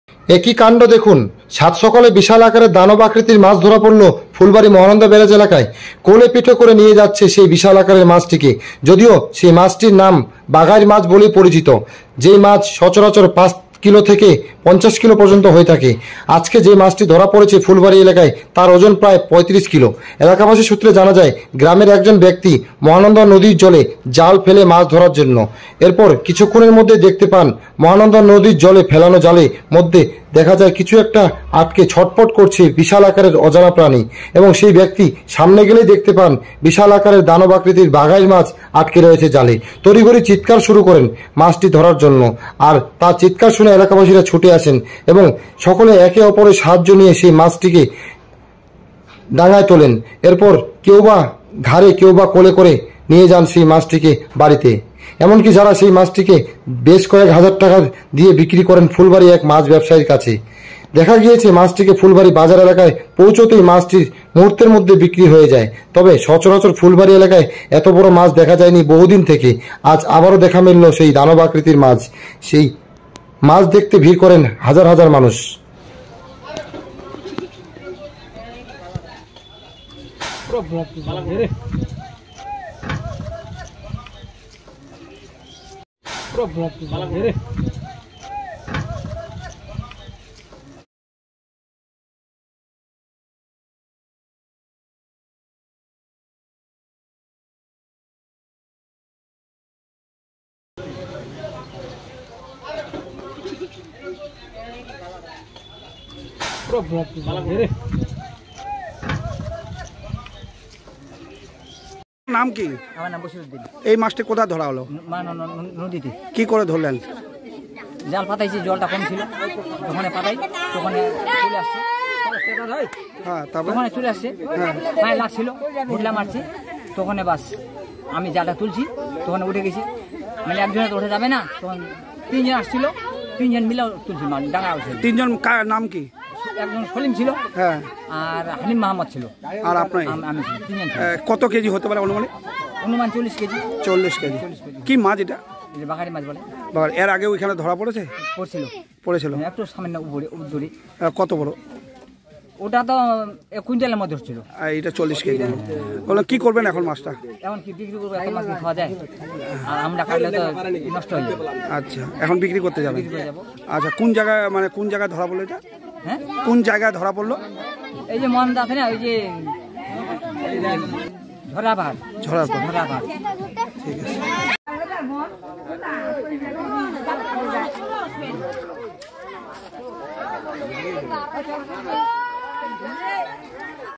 জালে আটকে ছটফট করছিলো এরপর দেখেন দানব আকৃতির বিশাল বাঘাইর মাছ বিক্রি হলো কয়েক হাজার টাকা দিয়ে
জালে আটকে ছটফট করছিলো
🔷এরপর দেখেন দানব আকৃতির বিশাল বাঘাইর মাছ
বিক্রি হলো কয়েক হাজার টাকা দিয়ে